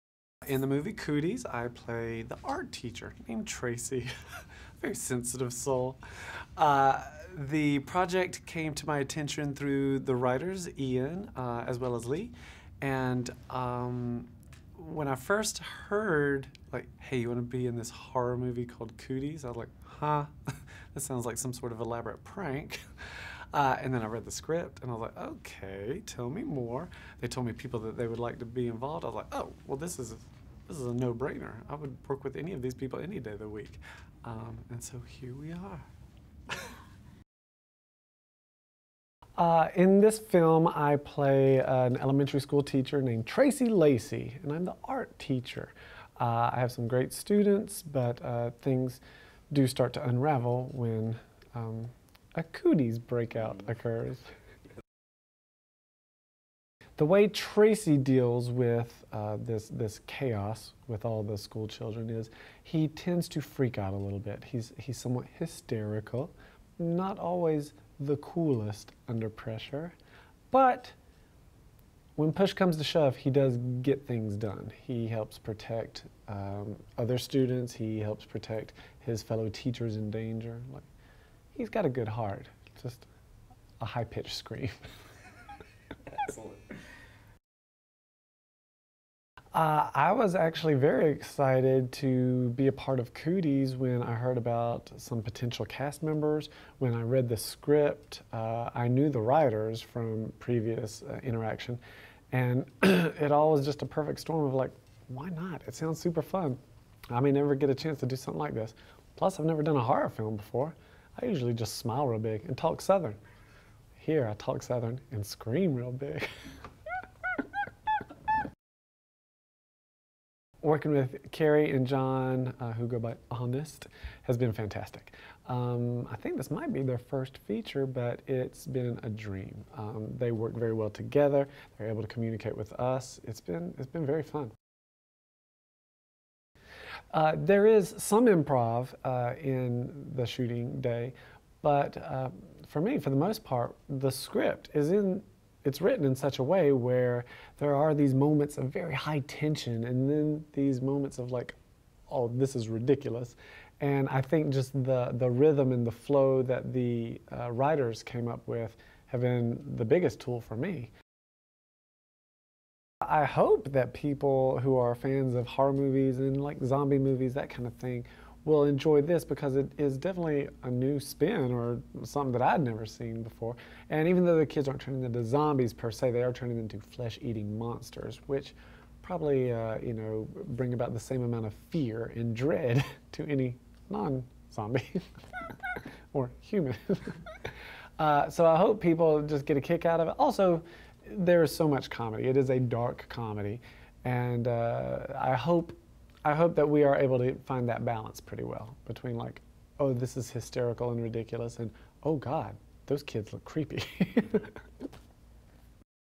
Cooties Interview - Jack McBrayer (2015) - Comedy Zombie Movie HD
A mysterious virus hits an isolated elementary school, transforming the kids into a feral swarm of mass savages. An unlikely hero must lead a motley band of teachers in the fight of their lives.